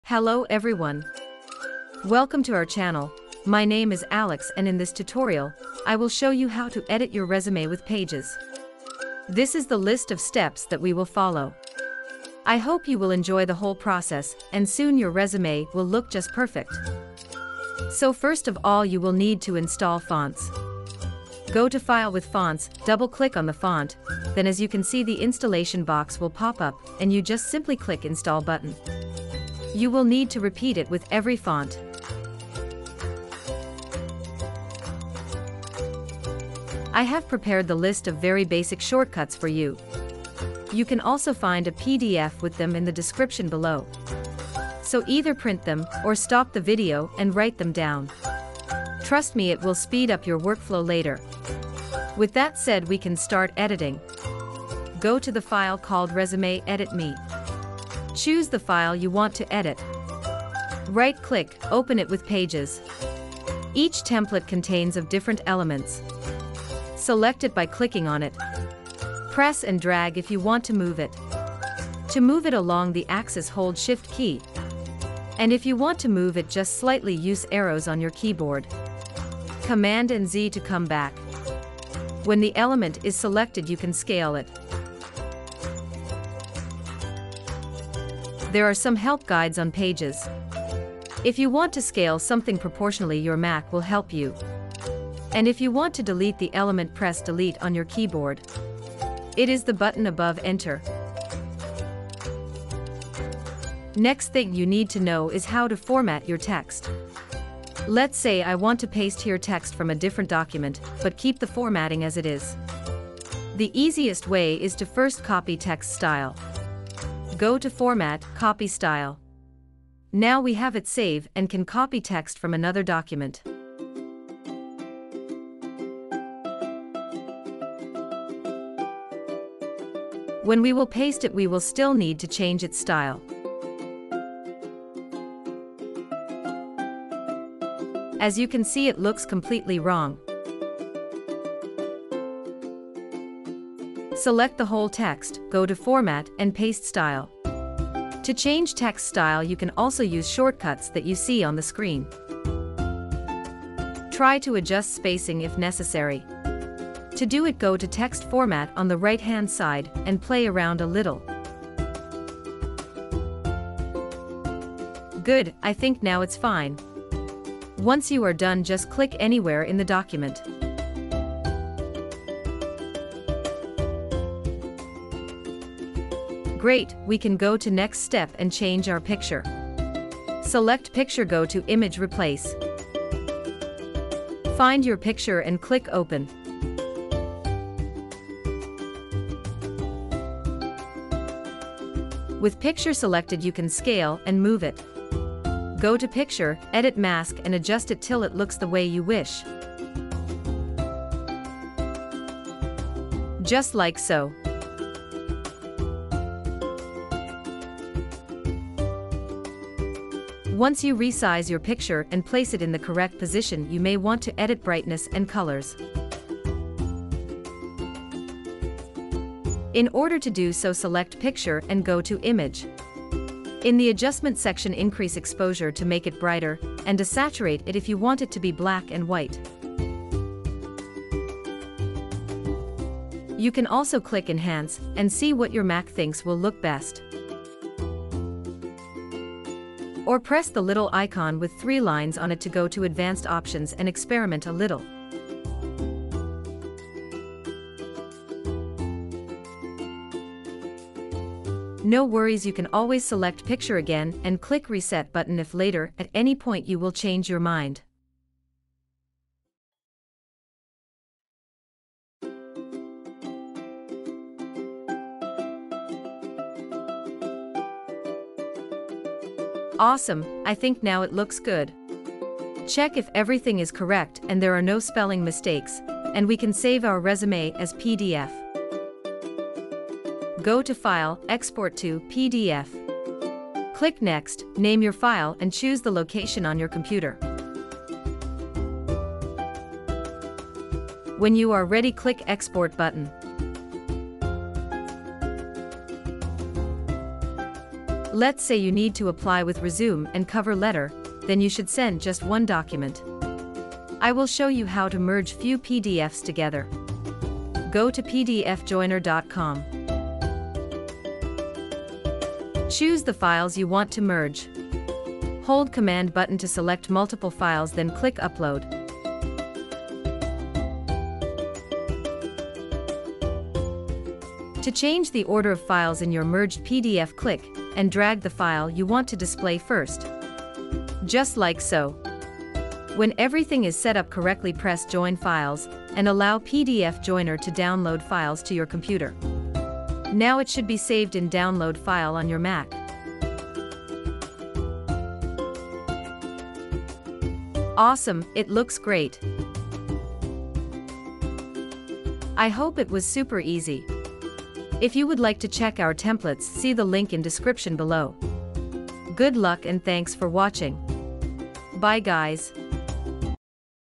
Editing resume template in Pages on MAC
Simple and easy guide for editing resume template on MAC Pages. We will help you find you dream job and go through recruitment process without stress.

Chapters:
0:19 Installing fonts on MAC
0:39 Shortcuts for Pages
0:55 Move, Resize, Delete elements
1:44 Text style format
2:54 Changing picture
3:10 Cropping picture
3:36 Editing picture
4:32 Saving Pages file as PDF
4:59 Merging multiple PDFs into one